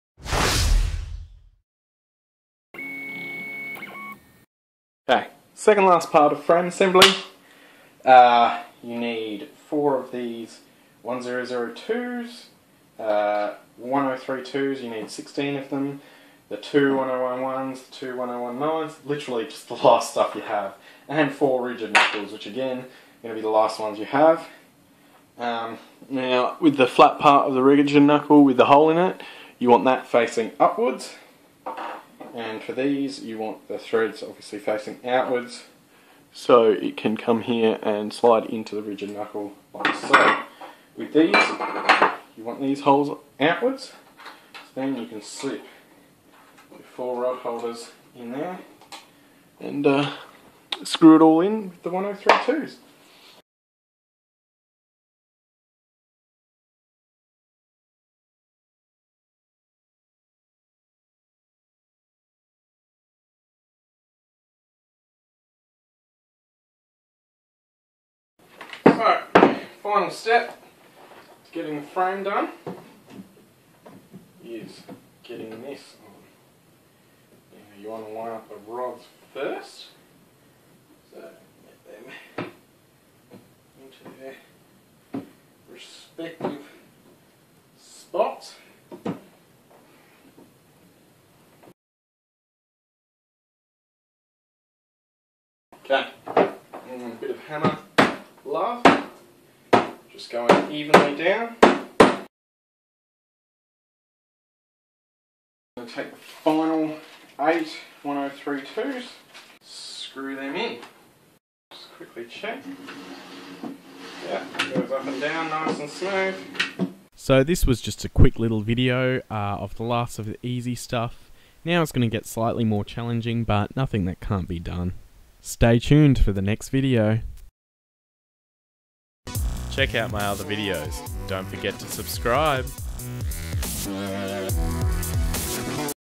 RigidBot Assembly - Part 5: Completing the Frame
Just a quick video where I complete the frame. Your Rigidbot should now be looking like a 3d printer. Get excited! You're close to being able to print (screwing in all the frame screws is the most time consuming part of the build)